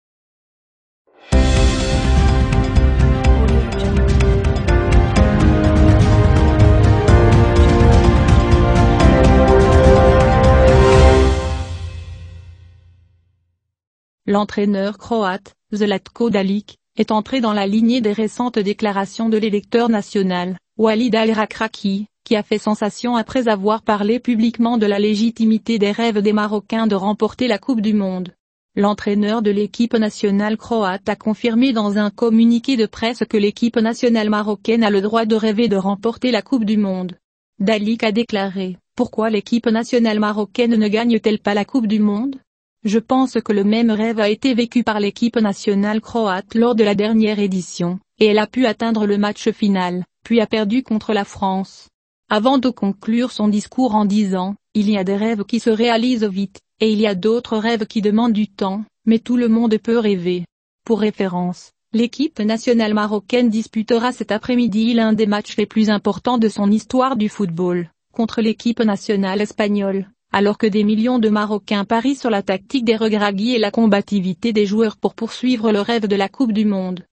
L'entraîneur croate répond aux déclarations de Rekraki sur le rêve du Maroc de remporter la Coupe
L'entraîneur croate répond aux déclarations de Rekraki sur le rêve du Maroc de remporter la Coupe du monde